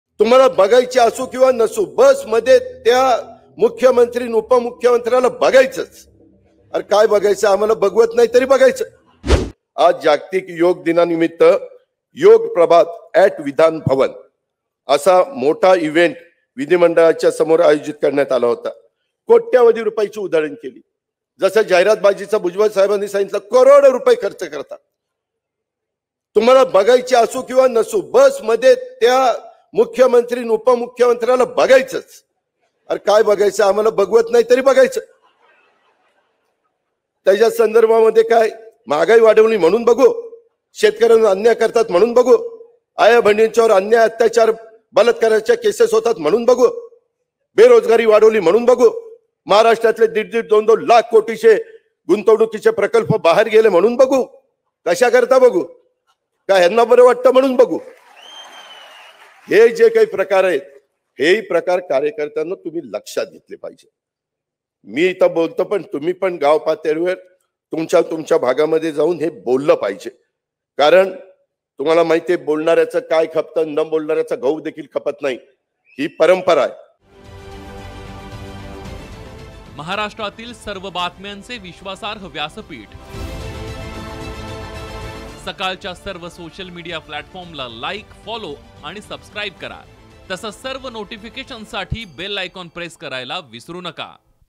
Ajit Pawar : Eknath shinde, Devendra Fadnavis यांना करोडो रुपयांच्या जाहिरातबाजीवरून सुनावलं
वर्धापन दिनानिमित्त राष्ट्रवादी काँग्रेसने आज मेळावा आयोजीत केला होता. यामध्ये अजित पवार यांनी कार्यकर्त्यांना संबोधित केले. यावेळी त्यांनी शिंदे-फडणवीस सरकारवर हल्लाबोल केला. मुख्यमंत्री - उपमुख्यमंत्र्यांची करोडोंच्या जाहिरातीवरून चांगलाच सरकारवर निशाणा साधला.